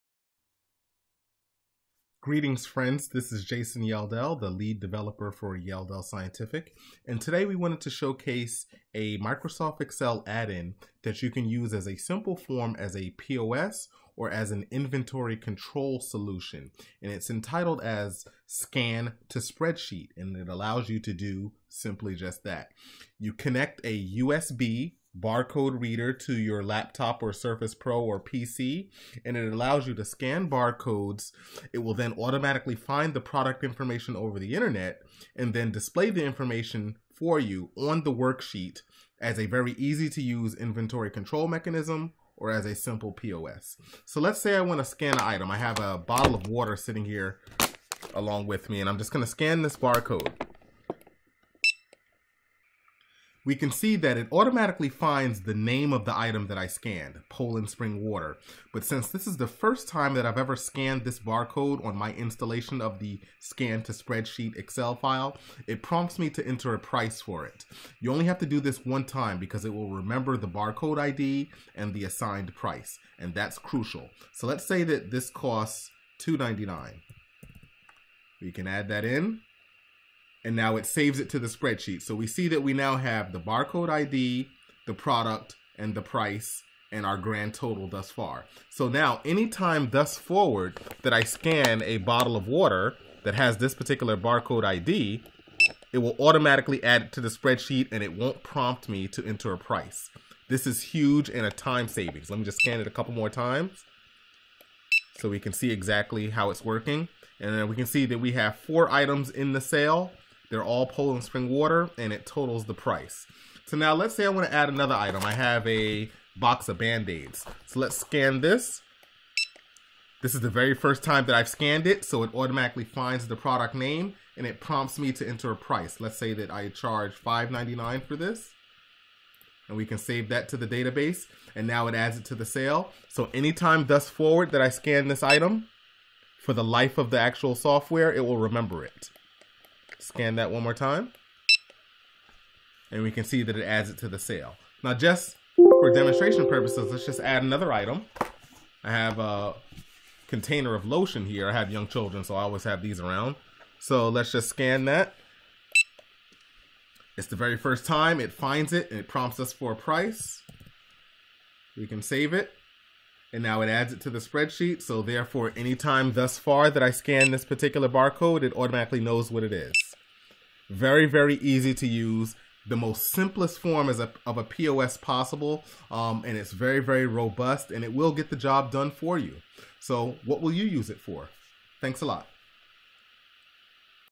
Excel Scan Barcodes to Spreadsheet - Simple POS/Inventory Management
Microsoft Excel Addin/Plugin allows you to scan barcodes and have product information automatically sent to spreadsheet along with price and a built-in database to house barcode IDS!

Manage Inventory
Use as a simple POS

What will you use it for?